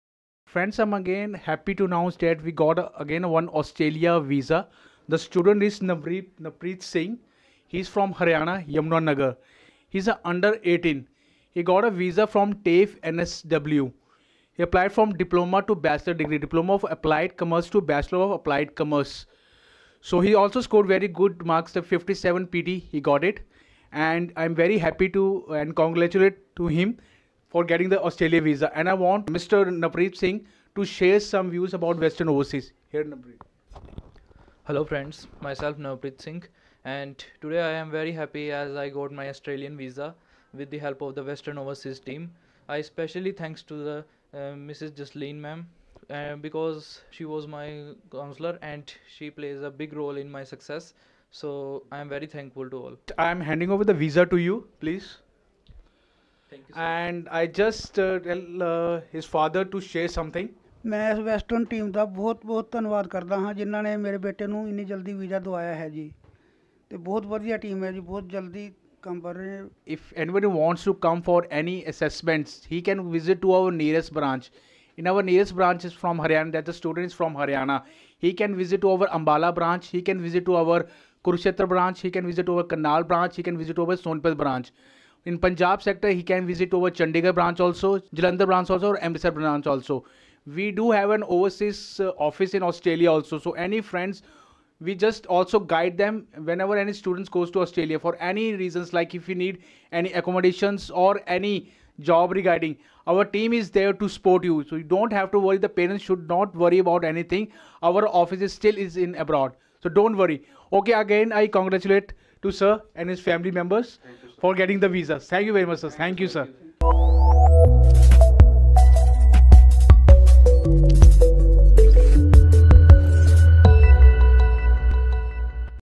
Under 18 Student Got Australia Student Visa For TAFE NSW
Australia Student Visa Approved in 10 days for TAFE NSW.
Congratulations to Mr. Navpreet Singh for his Australia Visa for Diploma Leading to Degree of Applied Commerce .

Student is 12th Passout with 65% & PTE - 57/56 Score.

Watch his success story.

Visit WesternOverseas Nearest Branch for a free counseling session.